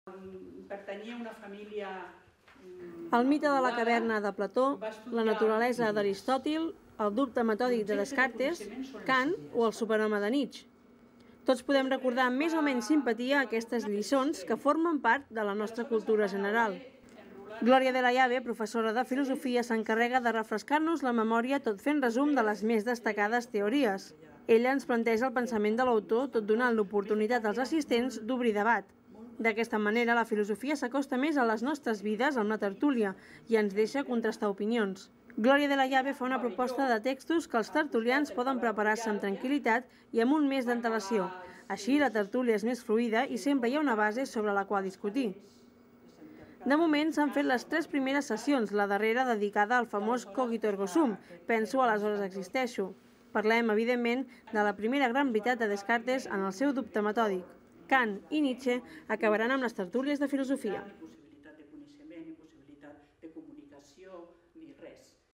Filosofia amb Glòria De La Llave
Molts ja han deixat enerere la Filosofia quan l'estudiaven a BUP o Batxillerat. Els actuals estudiants la tenen més fresca però sempre va bé fer una repassadeta dels autors més destacables. Per això, Glòria de la Llave ens ha proposat cinc tertúlies filosòfiques a la Biblioteca Marc de Vilalba.